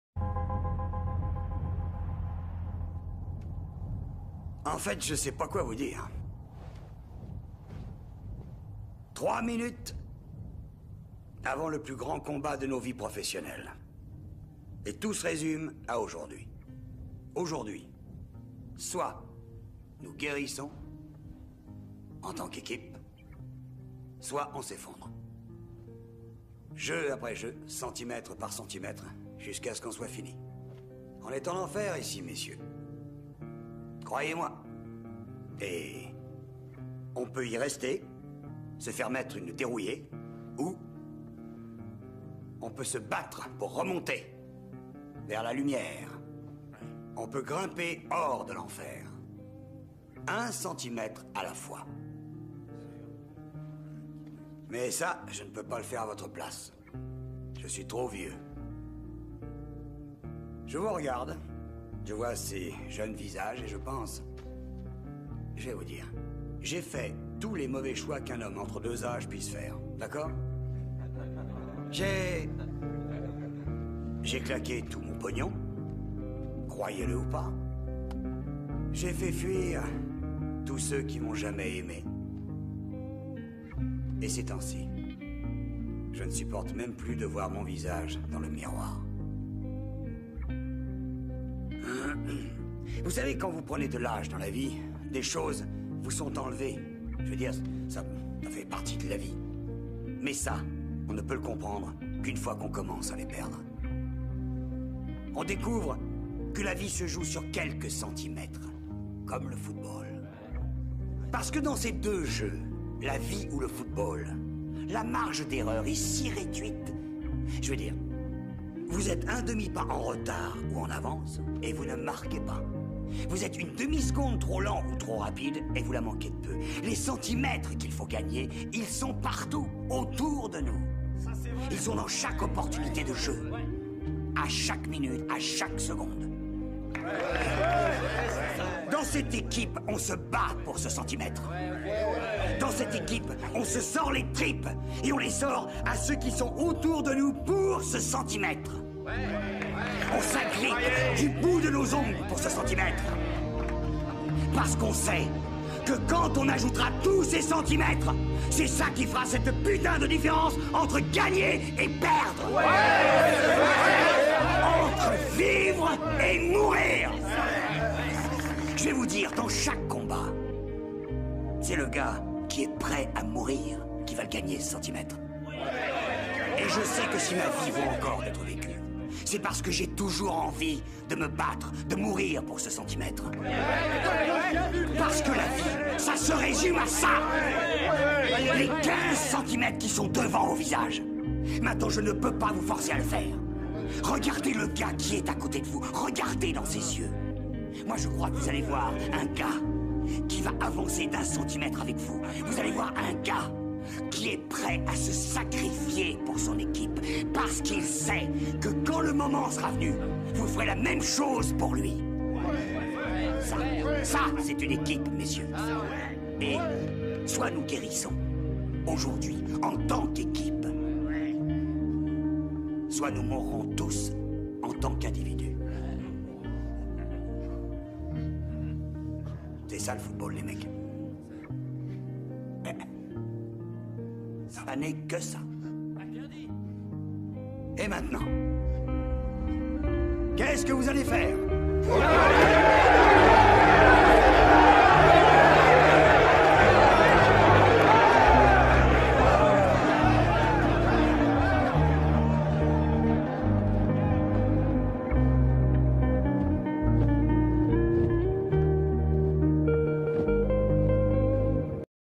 Discours de Zidane (avant un grand match)
Ce que pourrait être le discours de l'entraîneur Zinedine Zidane dans le vestiaire du Real Madrid avant un grand match de ligue des champions.